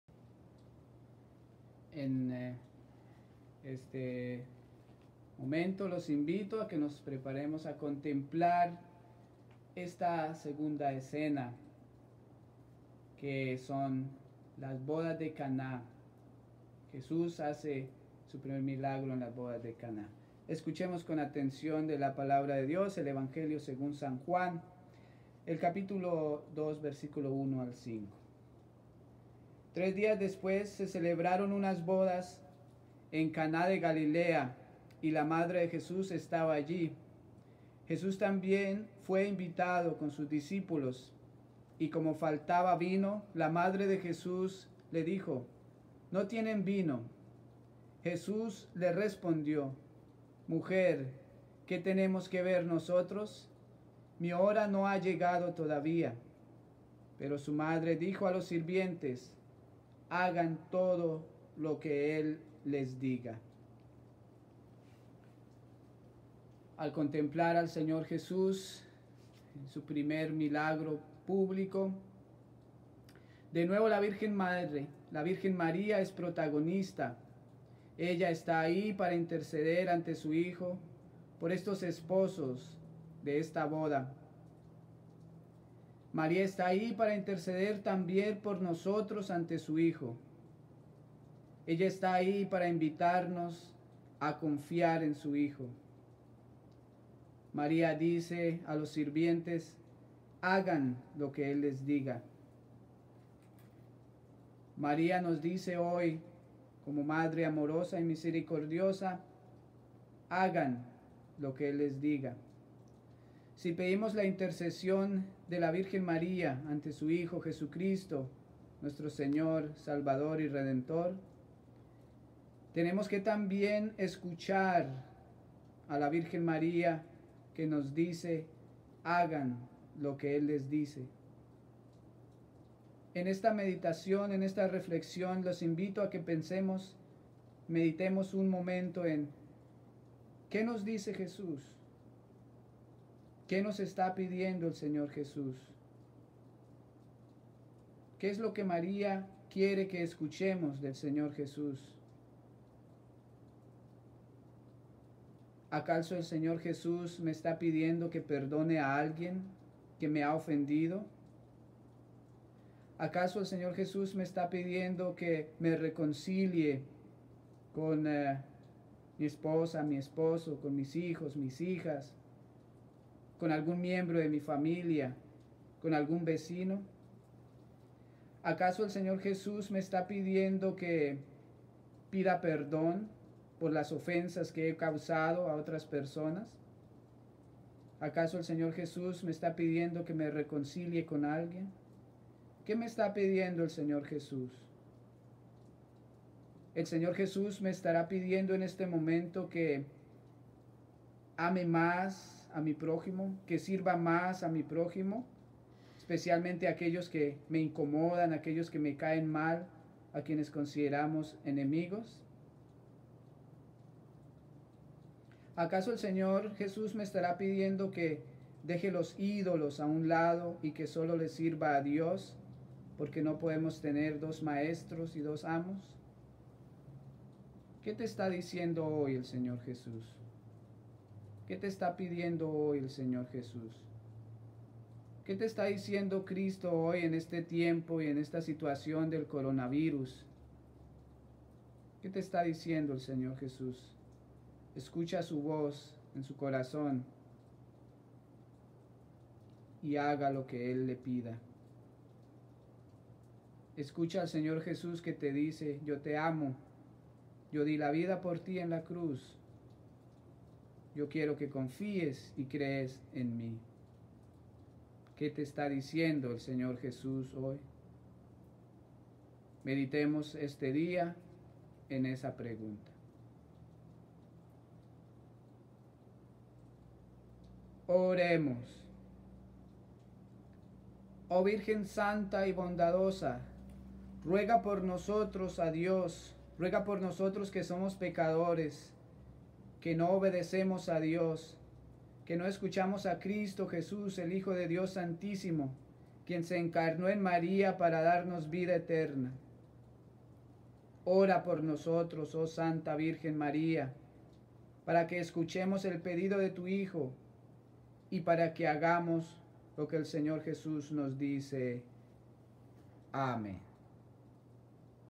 Retiro Espiritual (Video 2) - “La boda de Caná”
Padre Luis reflexiona y medita sobre las escenas de cada uno de los pasajes bíblicos que vemos representados en los vitrales de nuestro templo.